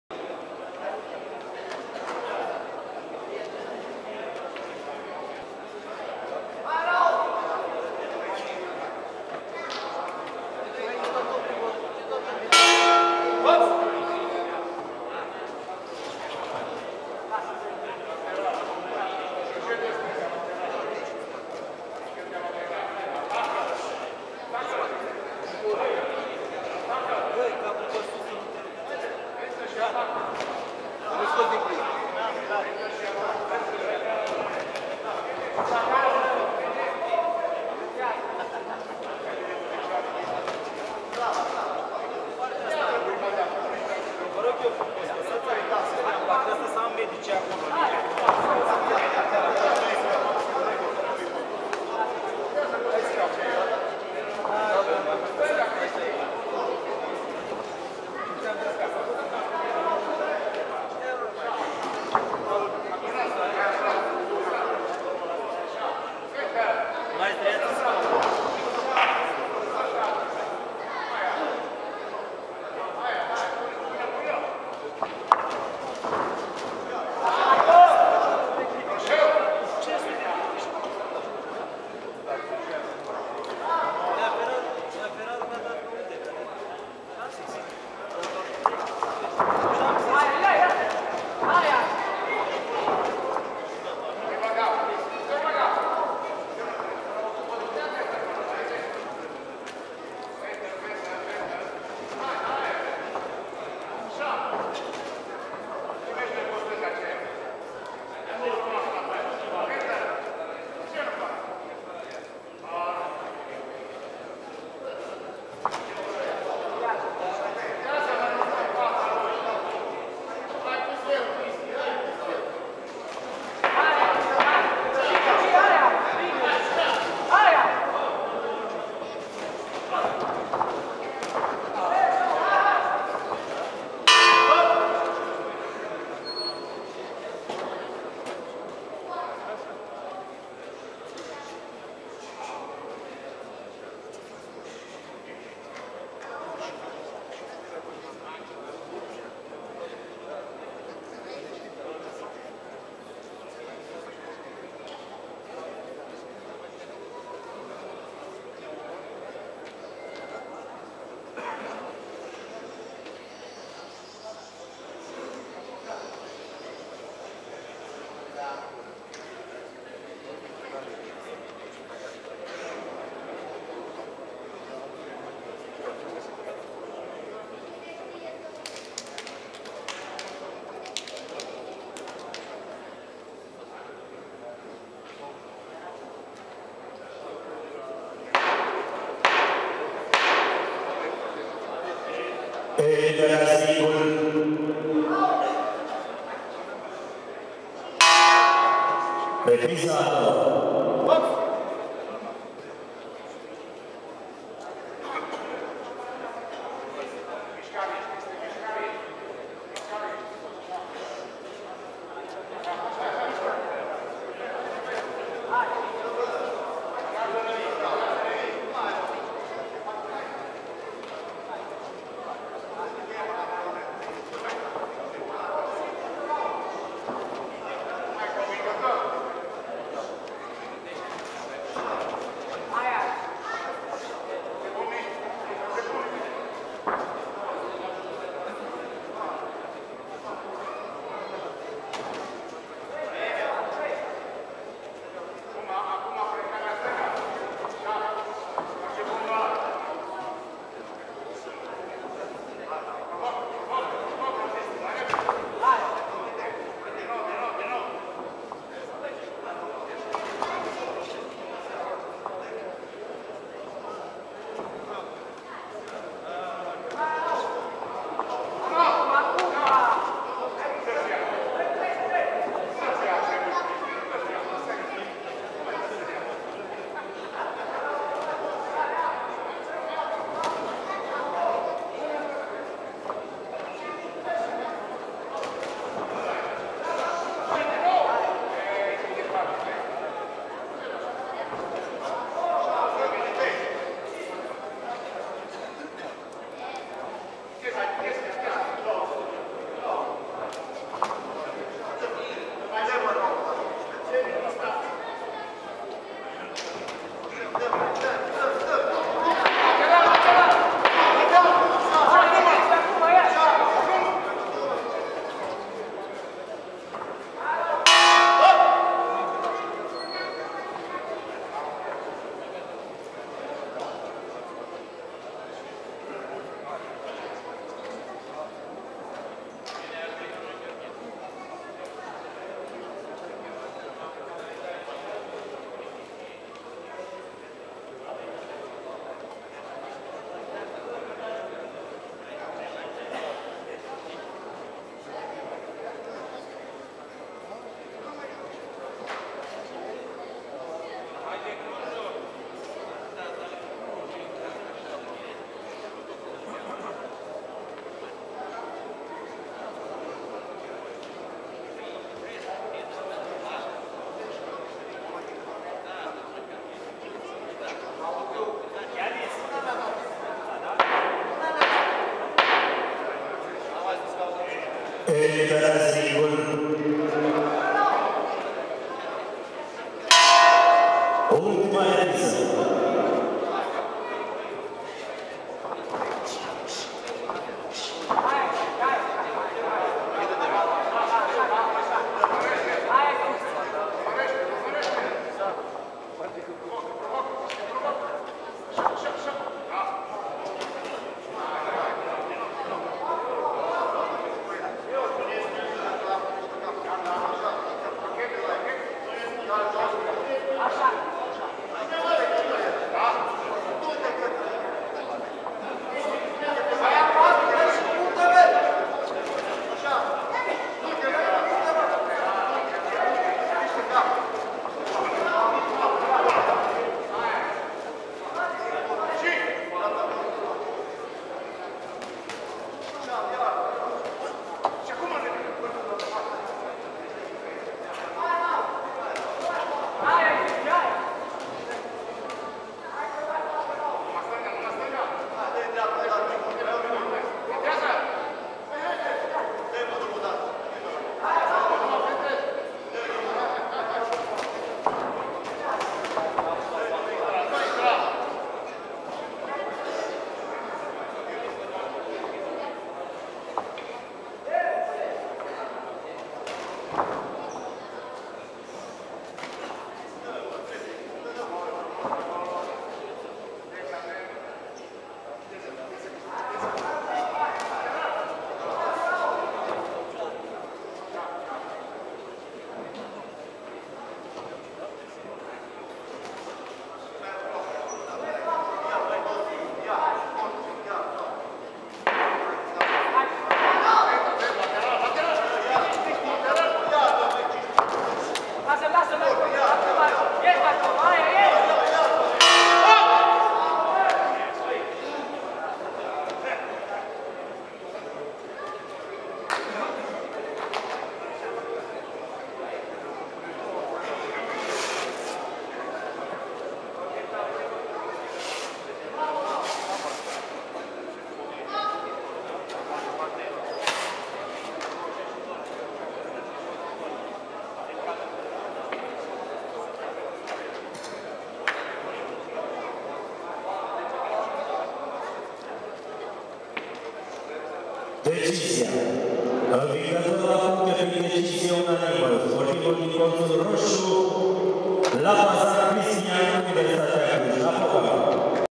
Lapazan Cristian vs Hrihor Raul
Campionatul National de Juniori 2019 Braila 46 kg Preliminarii